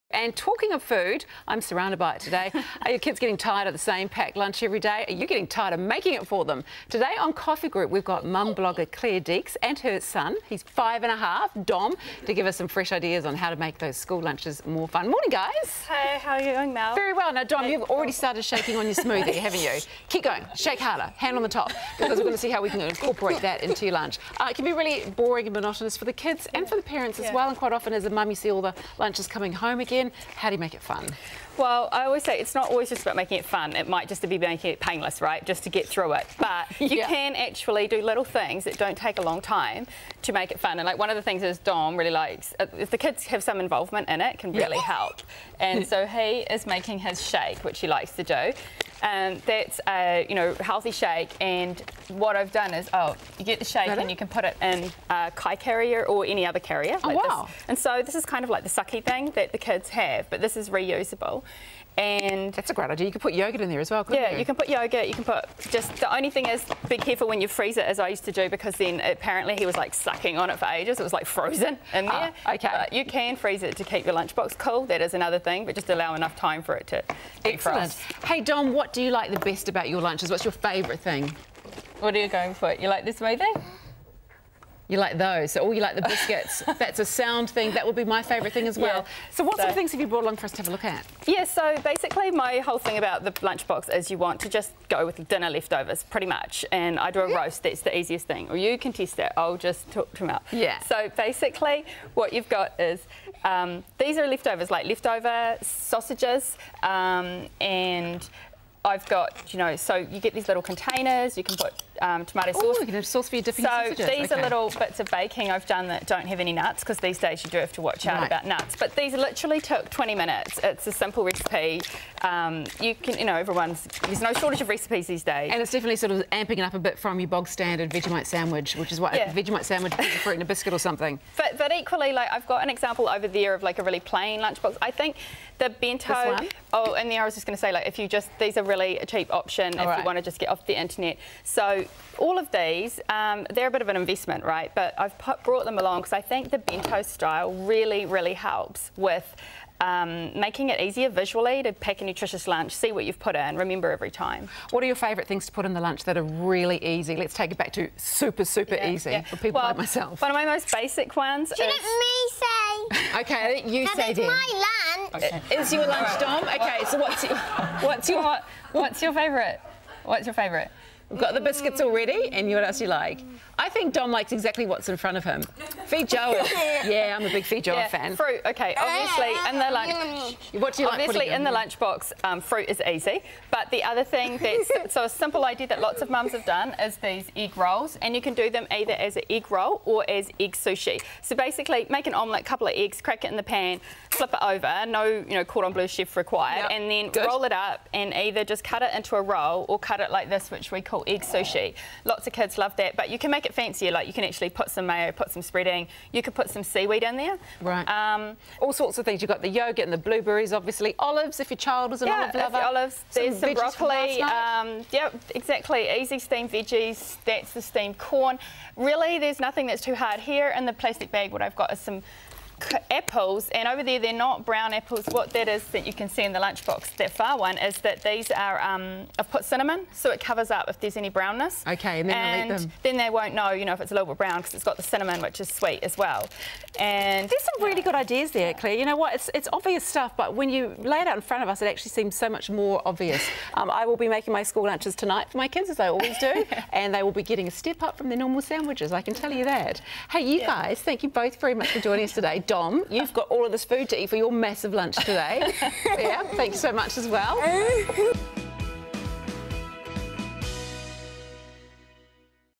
Taking school lunches from dull to Dynamic!
Popular "Dom's kitchen" blogger Claire Deeks and five year-old Dom have some cool ideas to make your kids lunchboxes less boring and more fun, fresh and healthy.